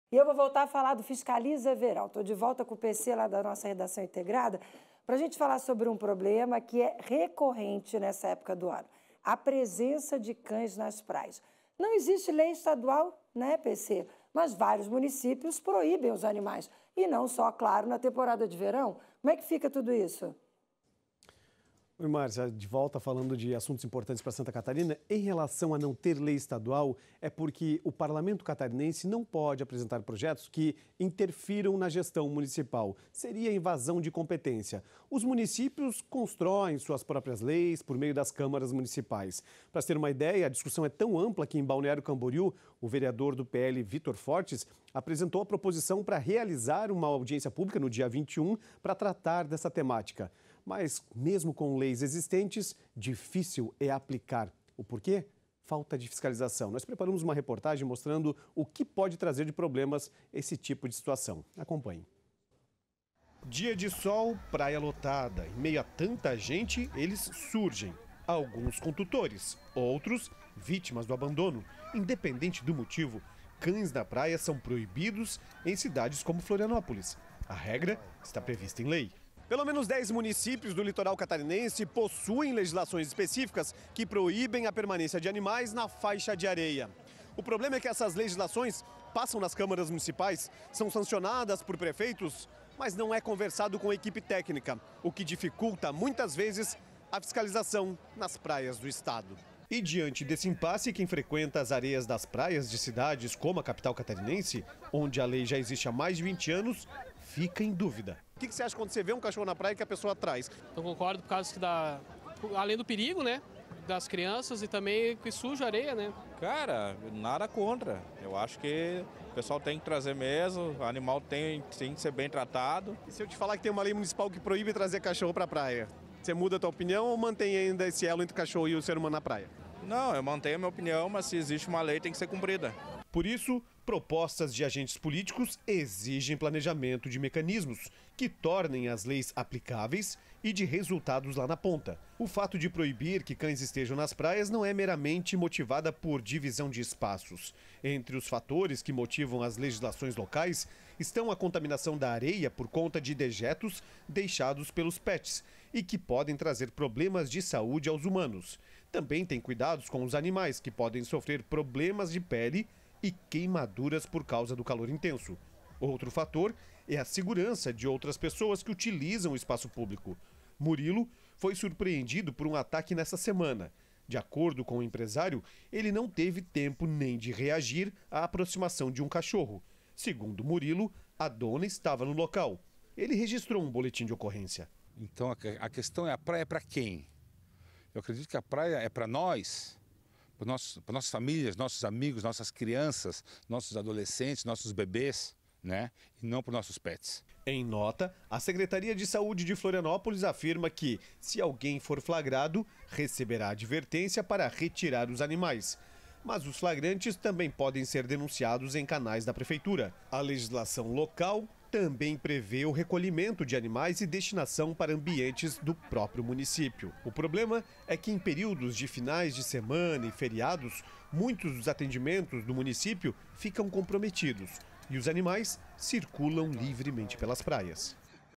Lei proíbe cães em praias de alguns municípios e causa polêmica
Em diversos municípios, a presença de cães nas praias é proibida por lei, o que tem gerado debates entre os frequentadores e moradores. Enquanto alguns defendem a medida por questões de saúde pública e preservação ambiental, outros acreditam que há formas de convivência harmoniosa com os animais nesses espaços. Paulo Cesar apresenta as opiniões dos envolvidos e explica como funciona a fiscalização durante a temporada de verão.